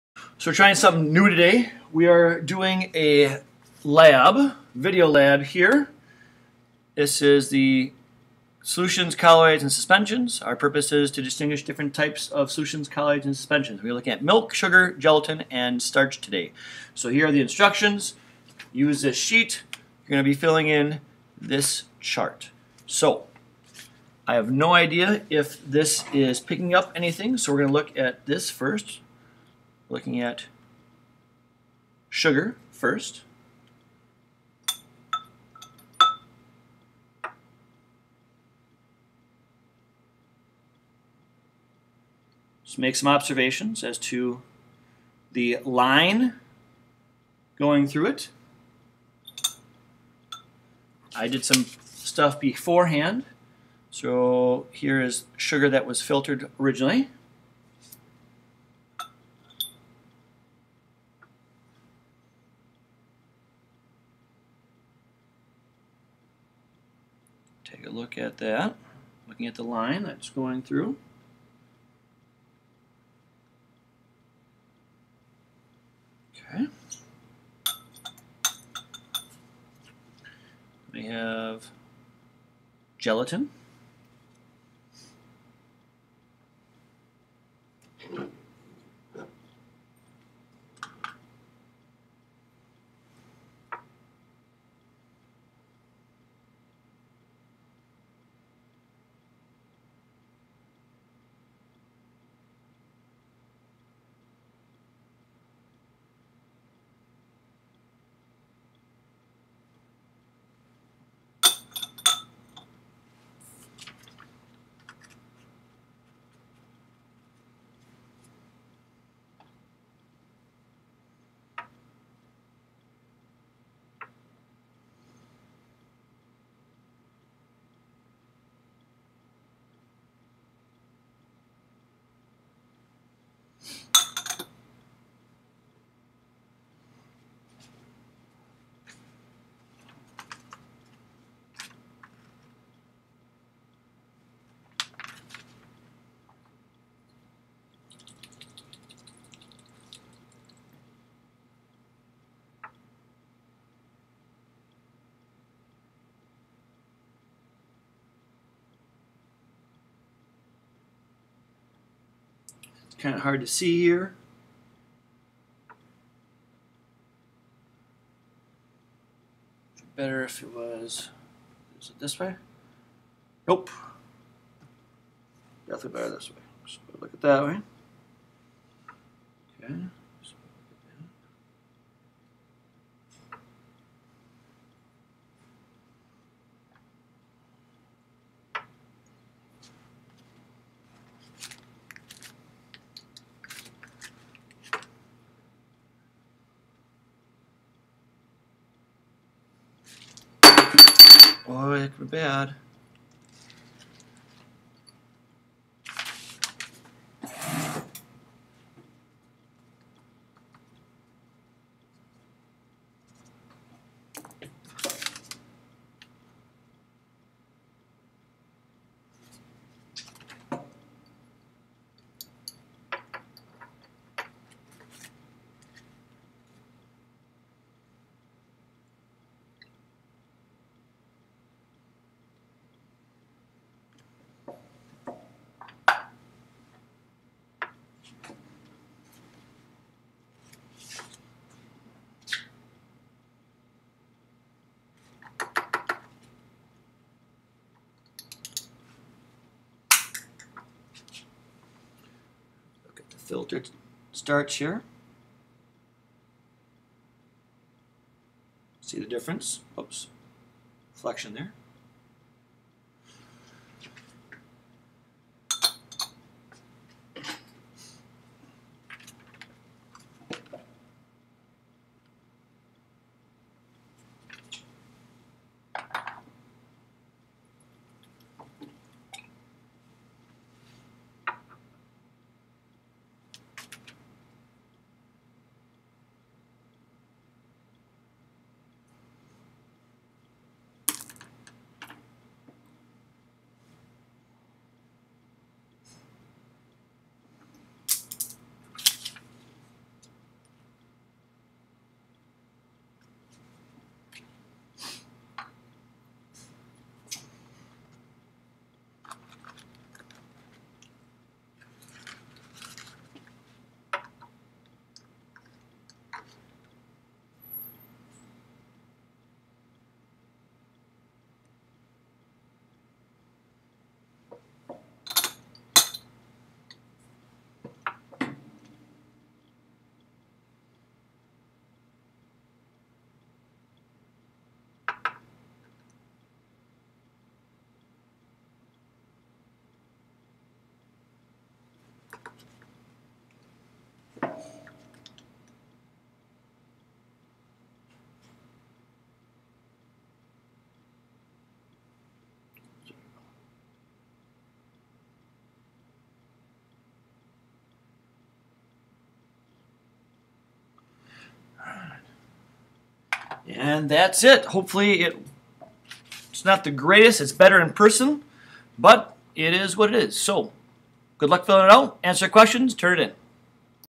Solution Colloid and Suspension video lab
Lab:         Purpose: To distinguish if different mixtures are solutions, colloids, or suspensions.